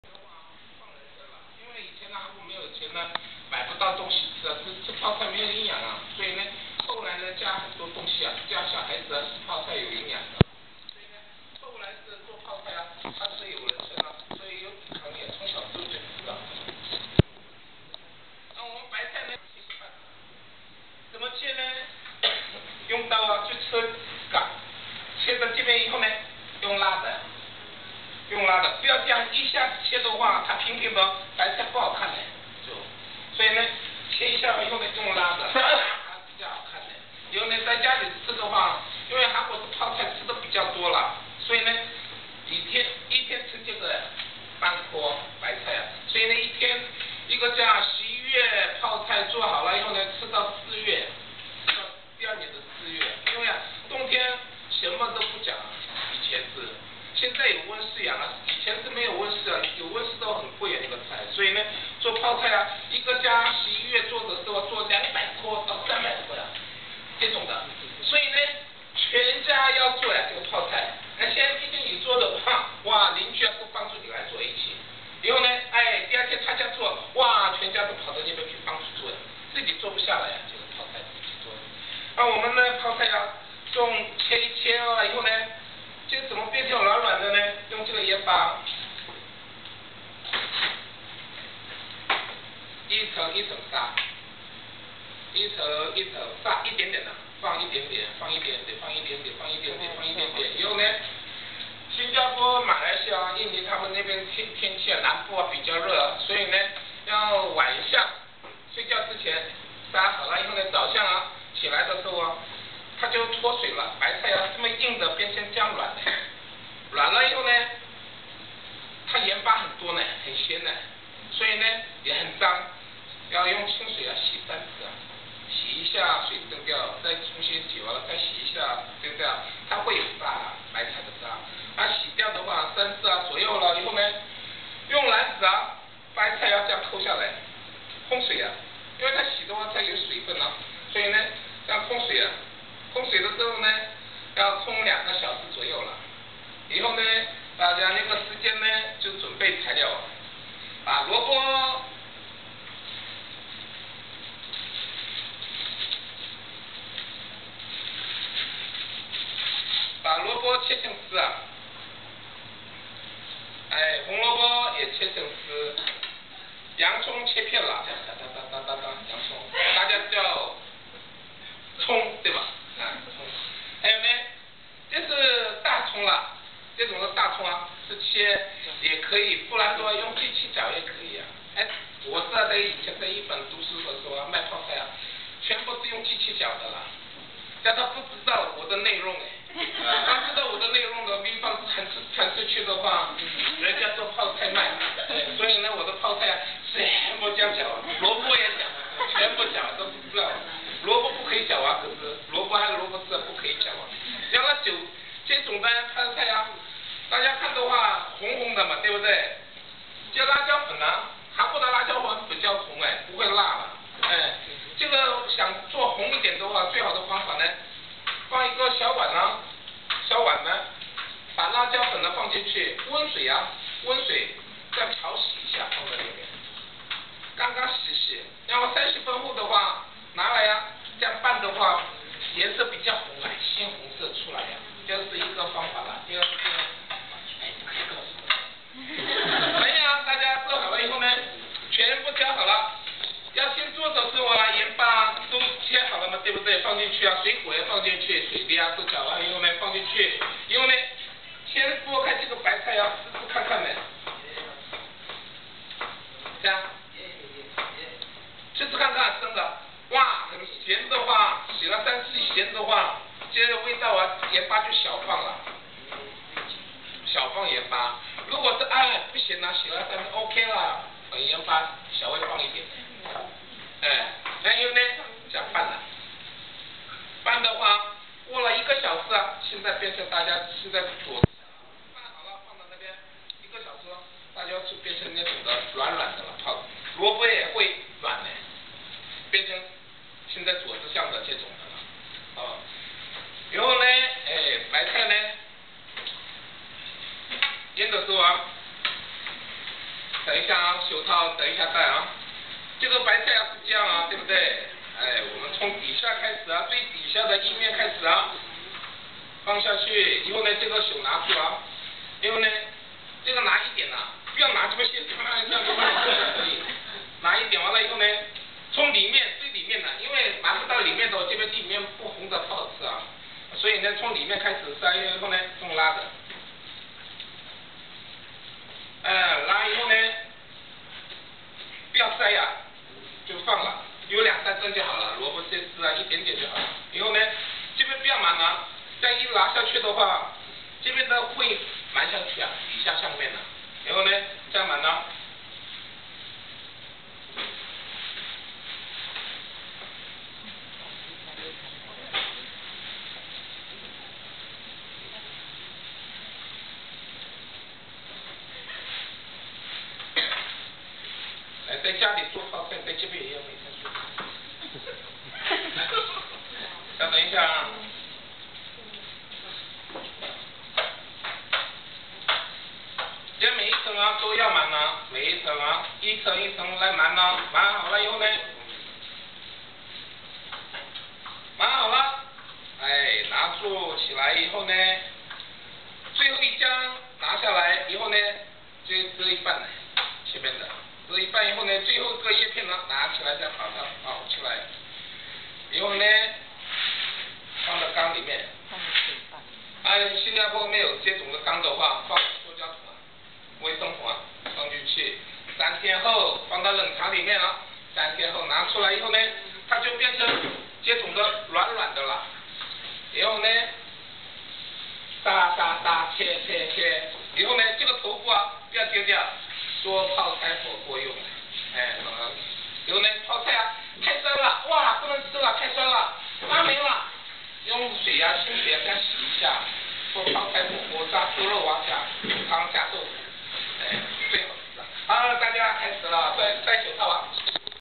2008 Korea 8D6N tour - Day 6, Kimchi Making Class
Kimchi Making "Make up" class which was supposed to be done on day 4.  Here you see is the tour guide teaching, not the people there!